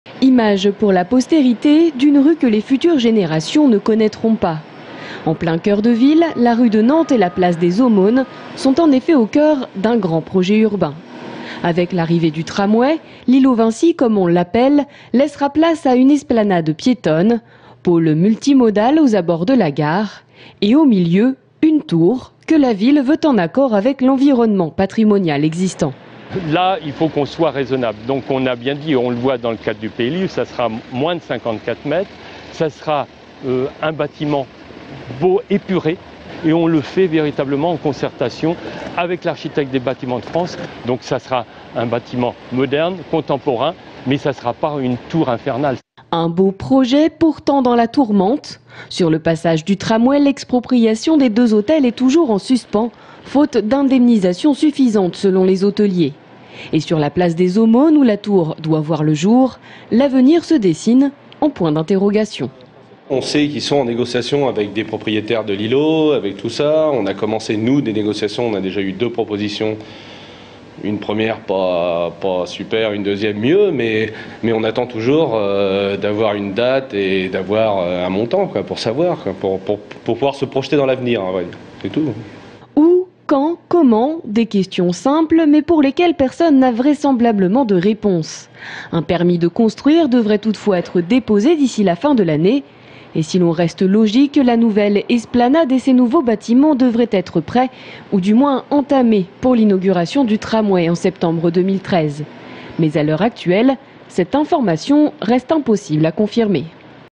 L'îlot Vinci rasé dans 10 jours (Tours)
Sites webs/blogs, copiez cette vidéo avec le bouton "Intégrer" !
 La destruction de l'îlot Vinci commencera le 11 Octobre. Elle permettra le passage du tramway.